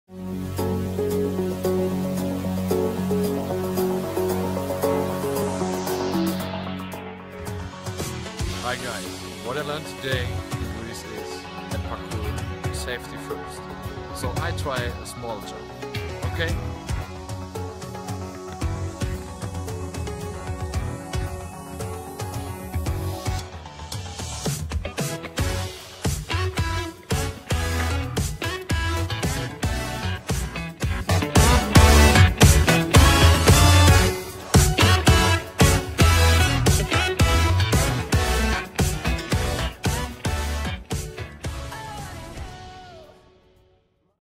Friedrichs first jump - Impressions from Greece
Part of our team is in Greece at the moment to ask Europeans from more than six countries about their European Stories.

However, there's more! The event "Find your balance through Parkour", which is Erasmus+ funded allows us to get to know new skills - such as shown here: Friedrich does his first attempts at Parkour!

More info and videos will follow later - as the sun is too beautiful to sit inside and edit videos all day ;)